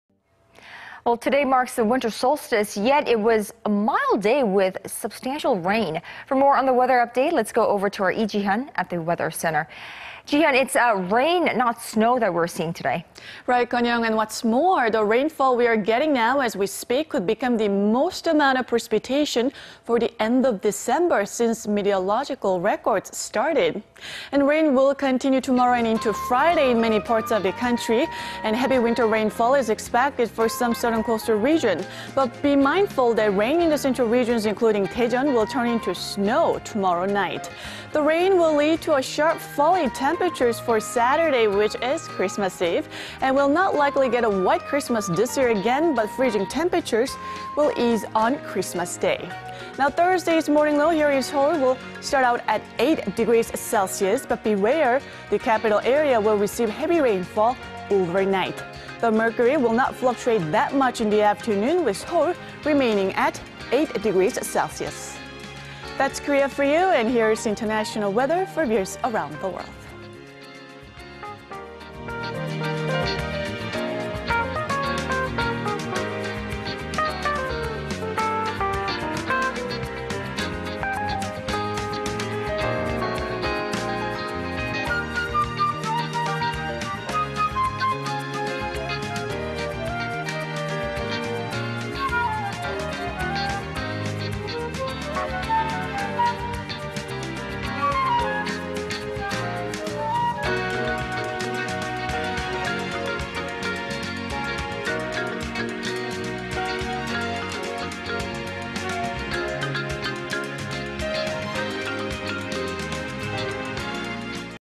Rain to linger until Friday in many parts
많은 지역 금요일까지 겨울비

  Today marks the winter solstice yet it was a mild day with substantial rain.
For more on the weather updates, let's go over to Lee Jee-hyun at the weather center.
Jee-hyun, it's rain not snow we're seeing today.

  That's right, and what's more, the rainfall we are getting now as we speak could become the most amount of precipitation for the end of December since meteorological records started.

 Rain will continue tomorrow and into Friday in many parts of the country. Heavy winter rainfall is expected for some southern coastal regions. Be mindful that rain in the central regions including Daejeon will turn into snow tomorrow night.

 The rain will lead to a sharp fall in temperatures for Saturday, which is Christmas Eve. And we will not likely get a white Christmas this year again but freezing temperatures will ease on Christmas day.

  Thursday's morning low in Seoul will start out at 8 degrees Celsius. But beware the capital area will receive heavy rainfall over night.

  The mercury won't fluctuate that much in the afternoon, with Seoul remaining at 8 degrees Celsius while Busan and Jeju Island will reach 15 and 13 degrees respectively.

That's Korea for you, here's look at the weather conditions around the world.